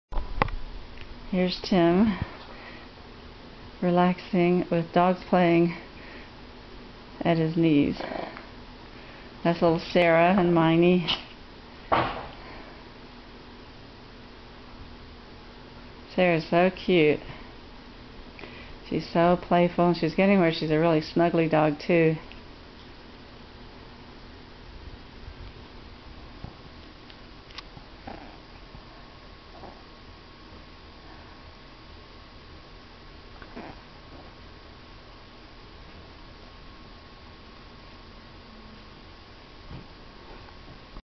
Yorkie puppies at 10 weeks - 3
has another episode in the life of three adorable teacup Yorkshire Terrier puppies. The puppies are going into their tenth week and Hoss and Peanut have left for their new homes, which means only Sarah was left for adoption. Two different adoption situations for Sarah have fallen through, but finally Sarah's "Mommy" came along. To follow the story of the puppies' lives, go